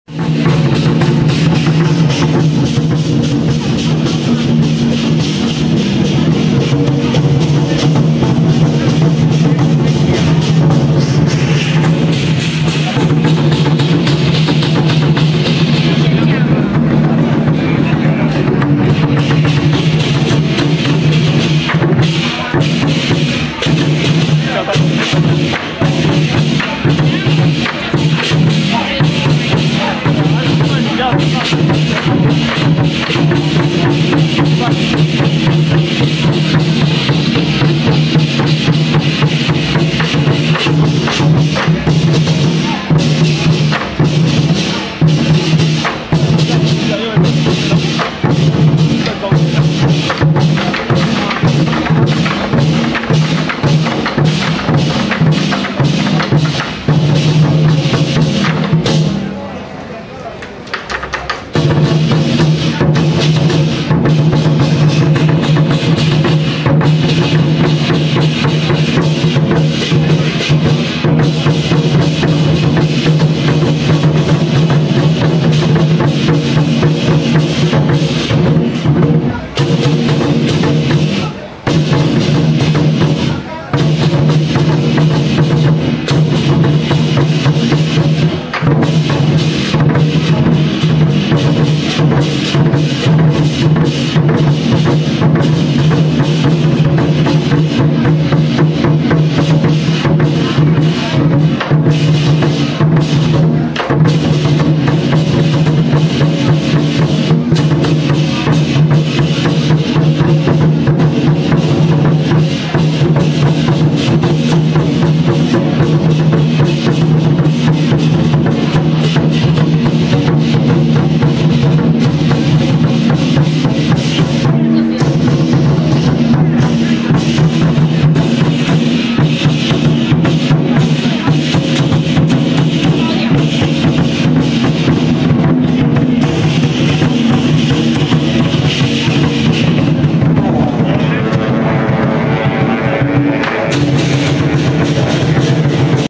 103板橋鎮發宮五年千歲十二王爺羅府千歲南巡謁祖進香回駕遶境02
{2}淡水震清會館戰鼓團